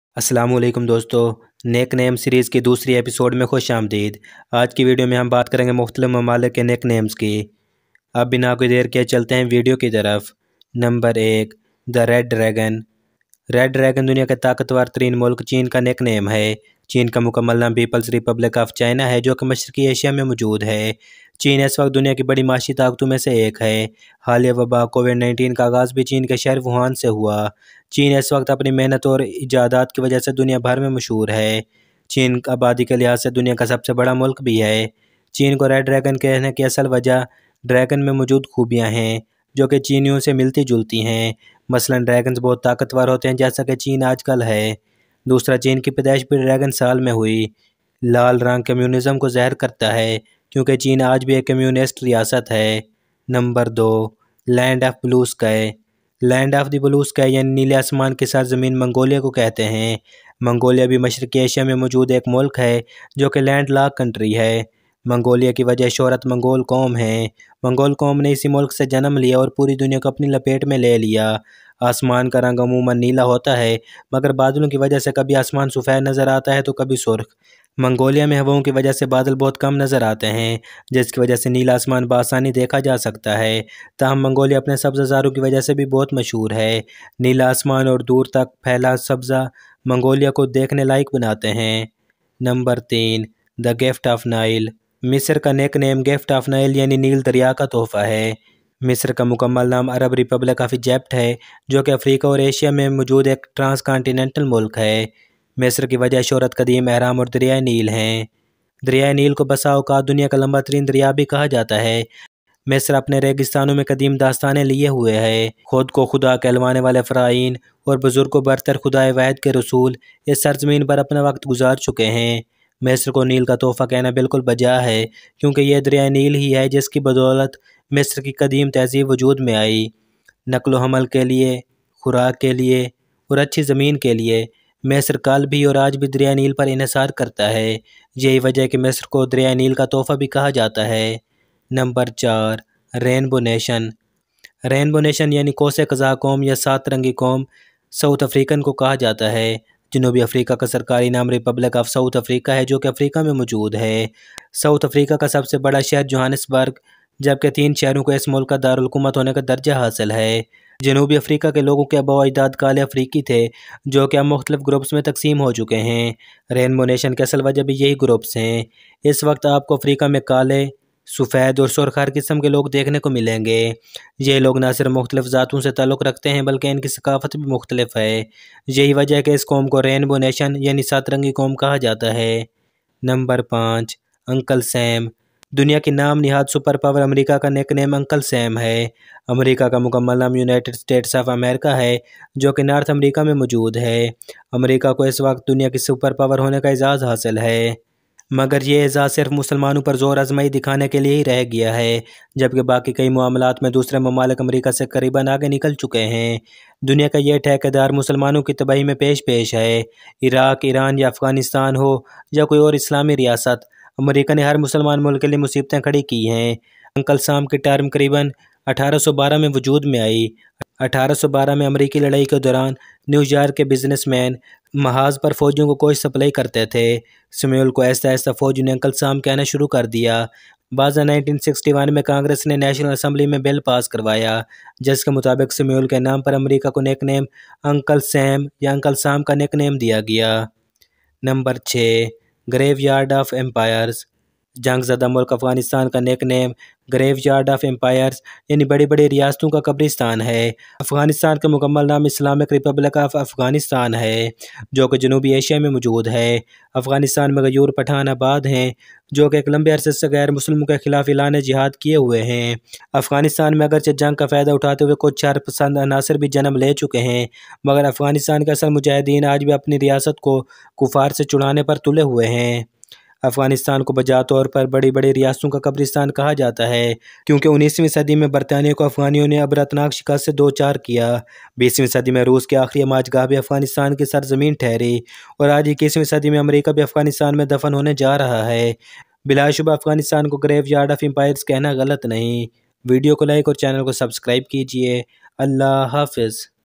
Nicknames | The Graveyard Of Empires.?
Nicknames is a new series on Point Out. Nicknames series created to find nicknames of different places, things and personalities. You can also find reason behind such nicknames.
This is the 2nd episode of series and the title of this video is

In this video, following points are include
The red dragon
China

Land of the blue sky
Mongolia

The gift of nile
Egypt

Rainbow nation
South Africa

Uncle Sam
America

The graveyard of empires
Afghanistan

Allah Hafiz.